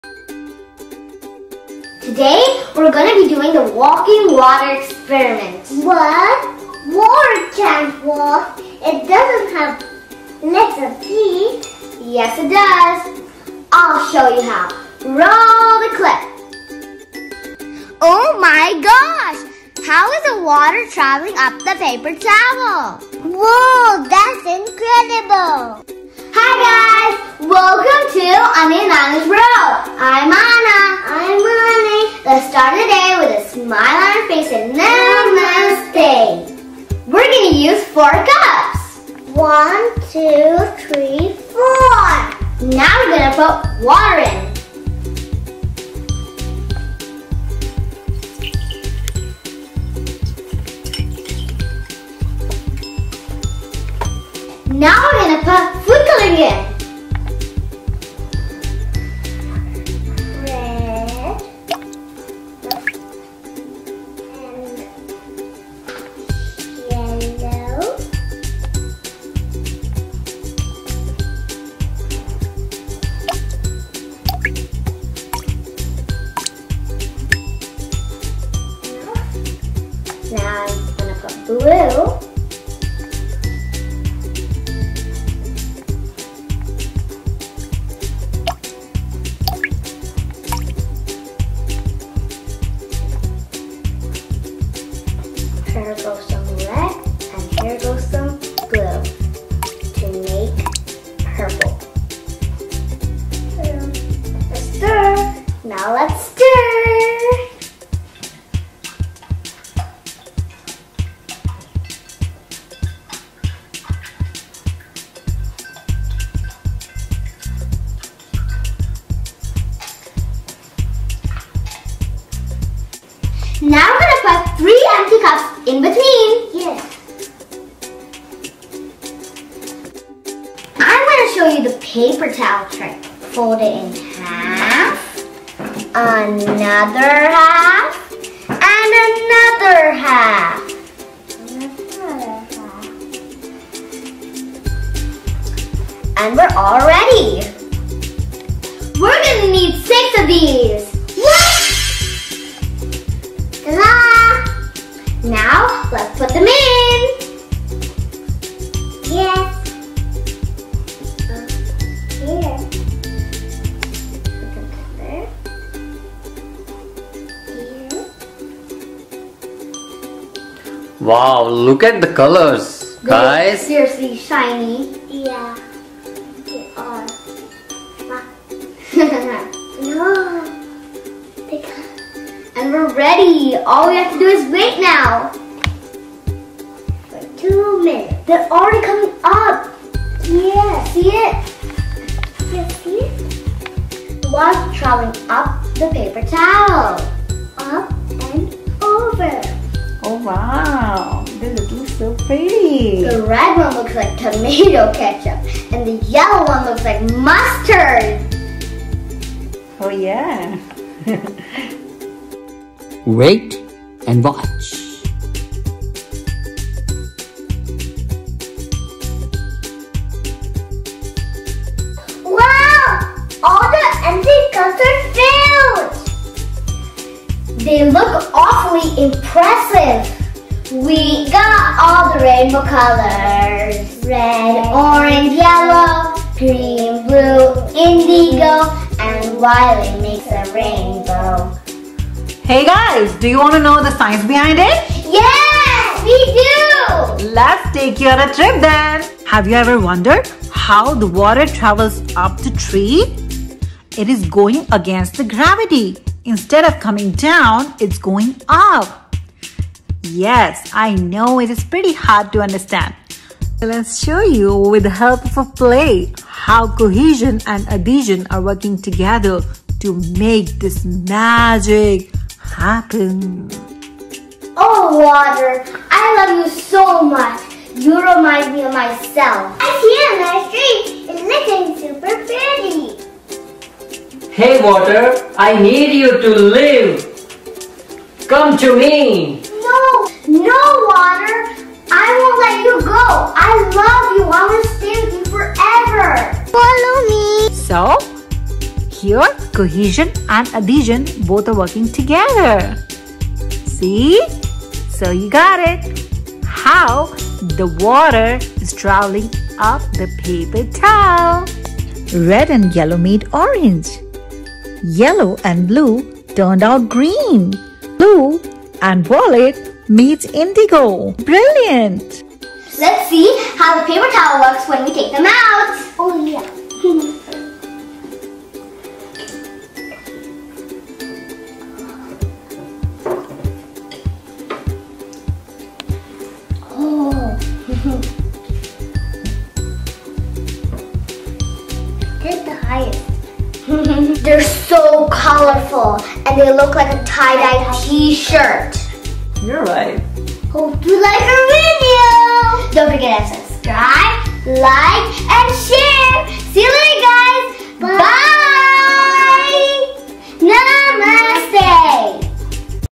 Walking Water Science Experiment | Anay and Aahna !
Walking water experiment for kids with Anay and Aahna
Science projects, science experiment for kids!!
Children will learn about how water travel up the paper towels through a process called Capillary Action.
This video will also explain how plants and trees are able to get water from the roots in the ground.
Fun and easy DIY experiment to do at home using everyday household items !